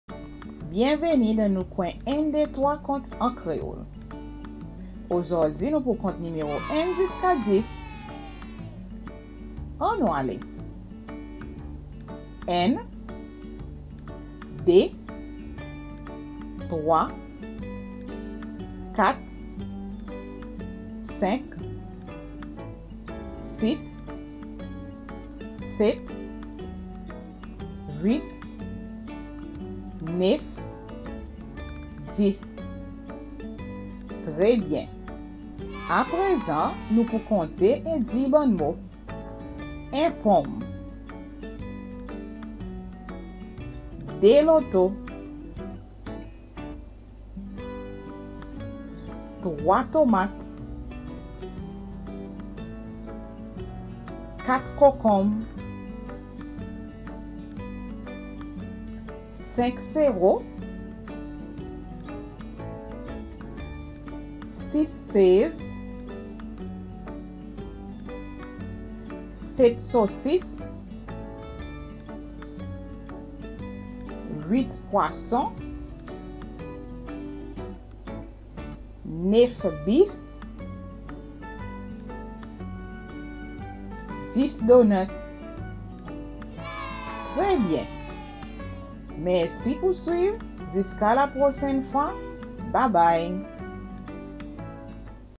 Count 1- 10 in Creole (Seychellois)
One of the three official languages of the Republic of Seychelles is Creole, and what better way to get started by learning how to count with 123 Count an Kreol.